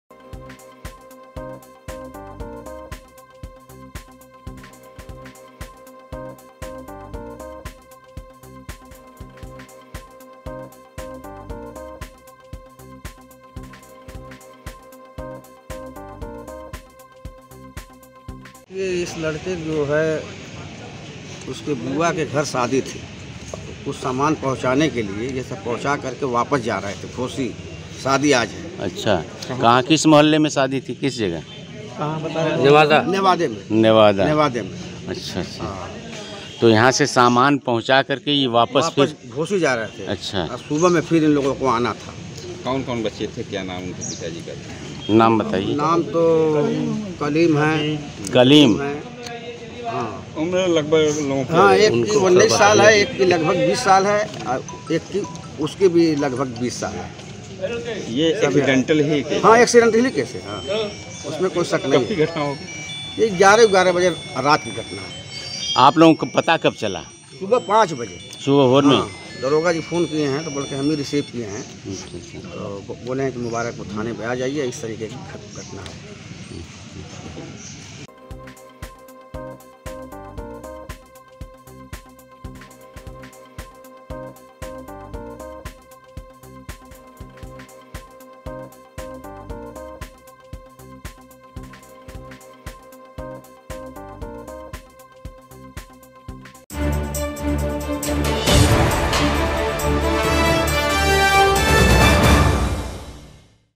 शादी की तैयारी में जा रहे तीन दोस्तों की गई जान, मचा हड़कंप
शादी के तैयारी में जुटे तीन युवकों के लिए अधिक रफ्तार में बाइक चलाना जीवन पर भारी पड़ गया। अनियंत्रित बाइक पेड़ से टकरानेे के कारण तीनों की मौत हो गयी। हादसे की जानकारी होने के बाद पुलिस मौके पर पहुंची और शव को पोस्टमार्टम के लिए भेज दिया। मृतकोें के परिवार में कोहराम मचा हुआ है। मृत तीनों युवक मऊ जिले के रहने वाले हैं।